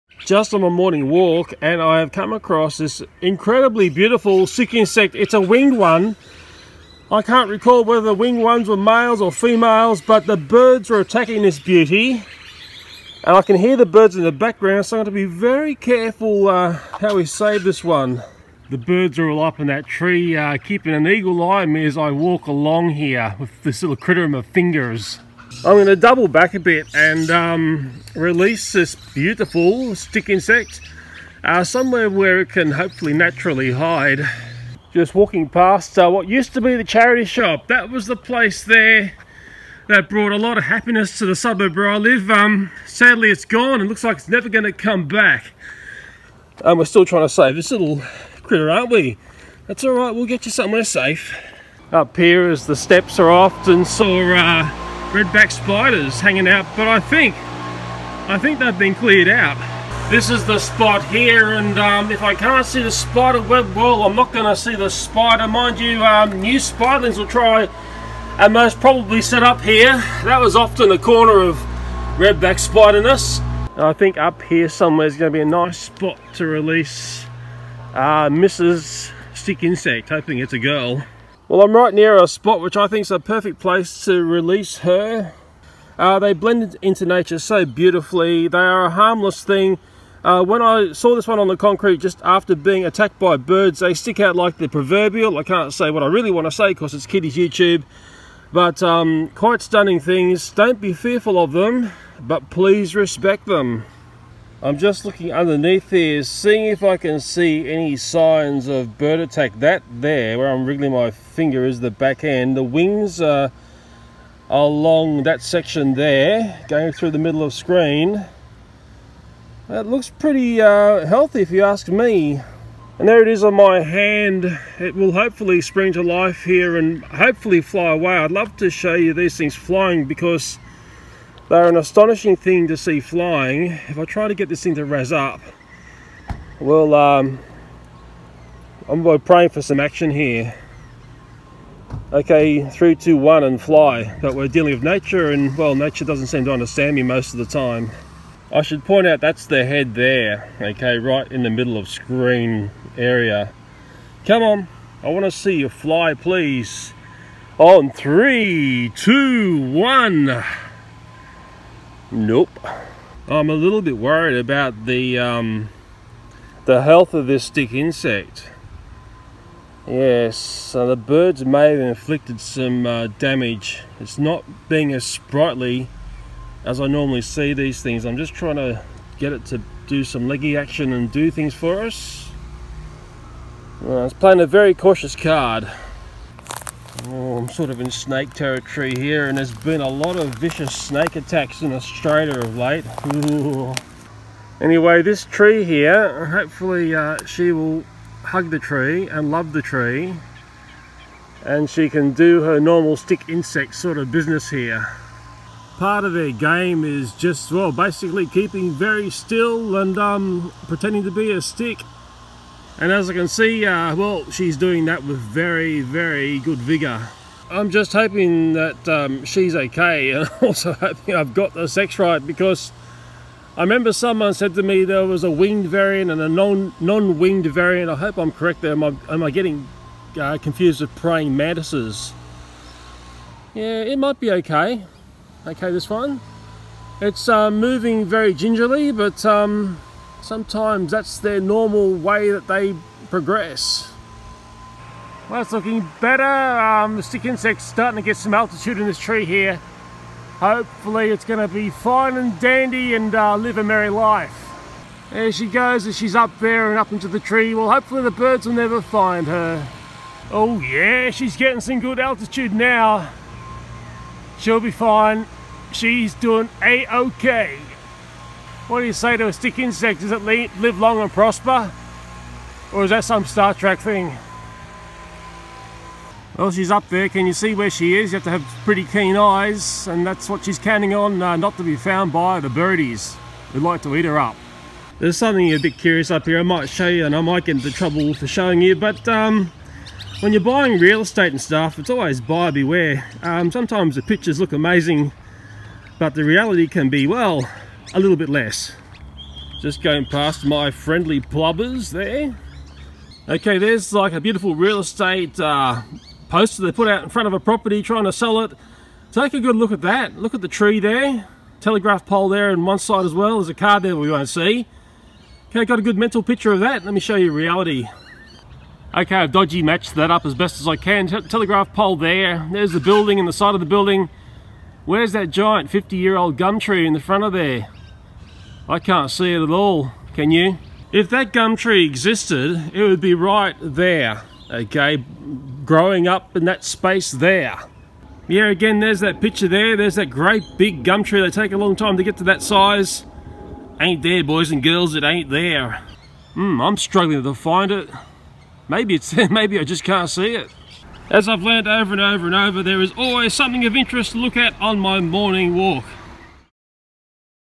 Stick Insect Saved & The Missing Tree
I found a stunning Stick Insect (looks like a stick so it can look like a tree) and this natural beauty was in a spot of trouble because the birds had found it. So lets re-locate her to a safe haven where the amazing camouflage can be far more effective. Then there is the problem of the missing 50 year old tree.

The Phasmatodea (also known as Phasmida or Phasmatoptera) are an order of insects, whose members are variously known as stick insects in Europe and Australasia; stick-bugs, walking sticks or bug sticks in the United States and Canada; or as phasmids, ghost insects or leaf insects (generally the family Phylliidae). The group's name is derived from the Ancient Greek φάσμα phasma, meaning an apparition or phantom, referring to the resemblance of many species to sticks or leaves. Their natural camouflage makes them difficult for predators to detect, but many species have a secondary line of defence in the form of startle displays, spines or toxic secretions. The genus Phobaeticus includes the world's longest insects.